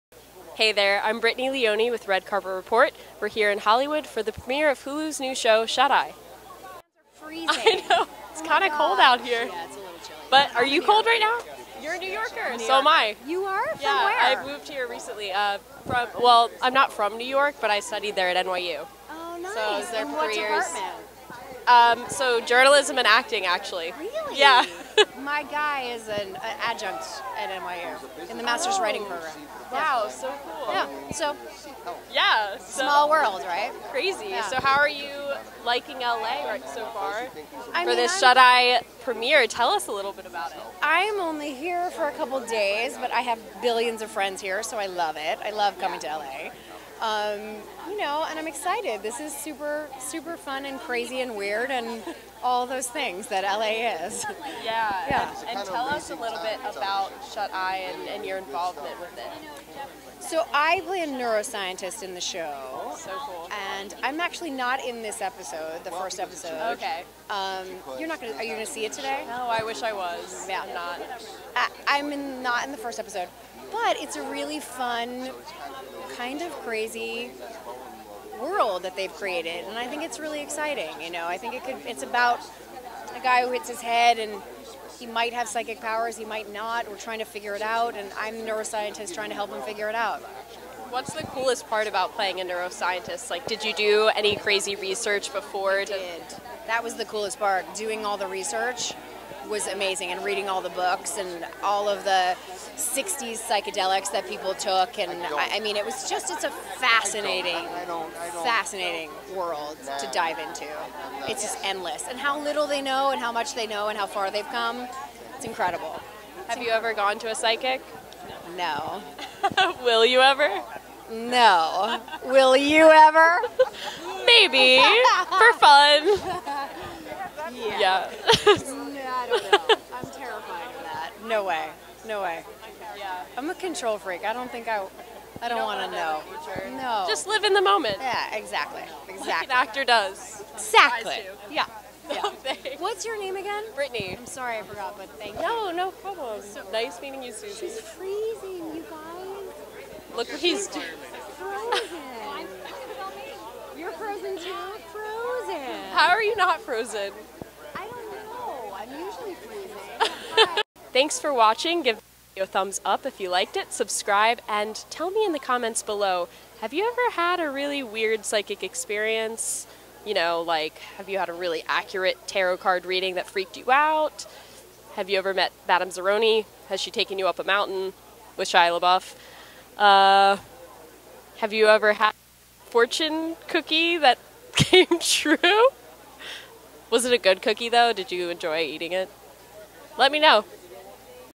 Susan Misner at the Red Carpet Premiere of "Shut Eye" on Hulu
Mingle Media TV and our Red Carpet Report team with host Brittany Leoni, were at Hulu's Red Carpet Premiere of Shut Eye, a new dark comedy, at the ArcLight Theatre in Hollywood.

All 10 episodes of Shut Eye begin streaming on Wednesday, December 7 on Hulu

About Shut Eye on Hulu
Shut Eye is a darkly comedic look at the world of Los Angeles storefront psychics and the 2,000-year-old history of belief and fraud behind them. The story revolves around Charlie Haverford (Jeffrey Donovan), a con man psychic whose worldview is rocked when he begins to have real visions.